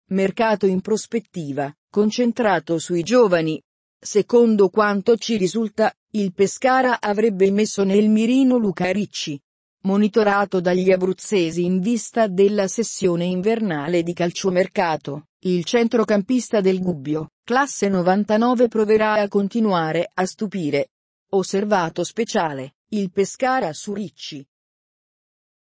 ESCLUSIVA TB - Pescara, piace un centrocampista dalla C
Mercato in prospettiva, concentrato sui giovani: secondo quanto ci risulta, il Pescara avrebbe messo nel mirino Luca Ricci. Monitorato dagli abruzzesi in vista della sessione invernale di calciomercato, il centrocampista del Gubbio classe '99 proverà a continuare a stupire. Osservato speciale, il Pescara su Ricci...